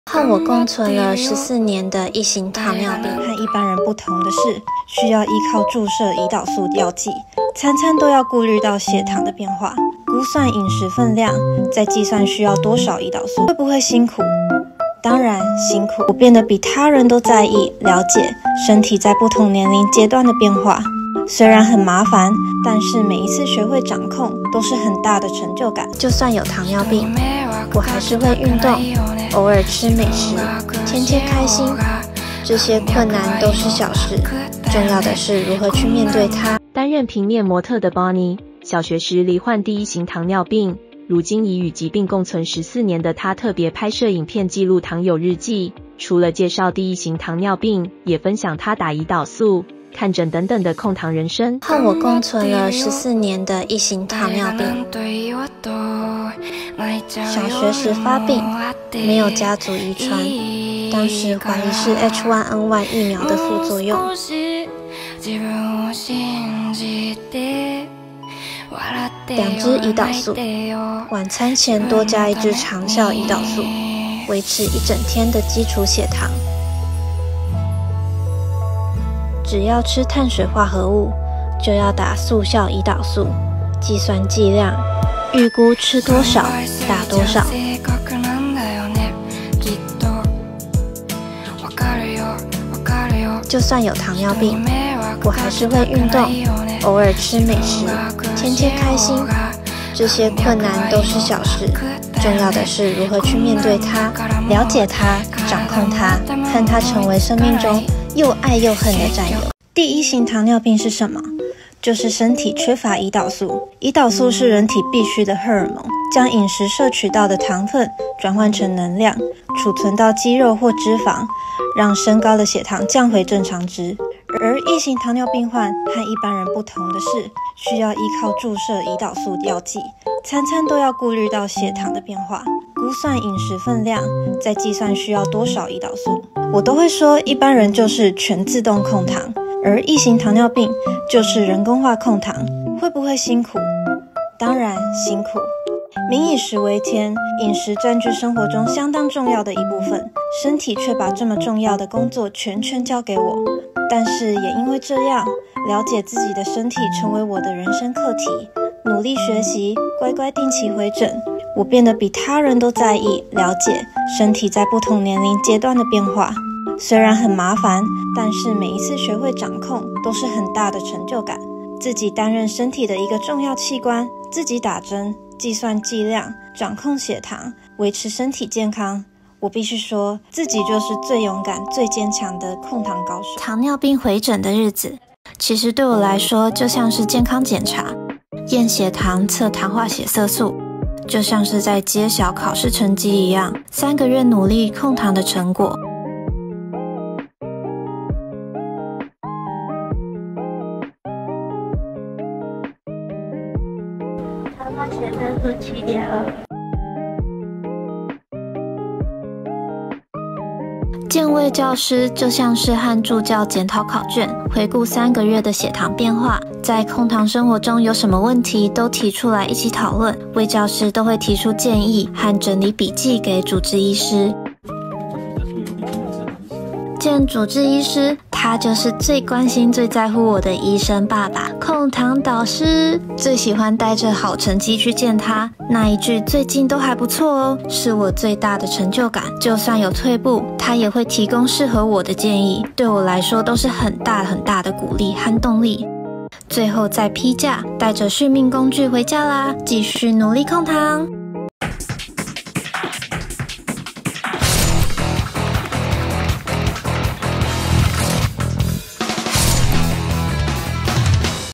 正妹模特罹患糖尿病14年 打針、回診正面看待「控糖人生」｜三立娛樂星聞
IG / Bonnie 邦妮 寶妮 授權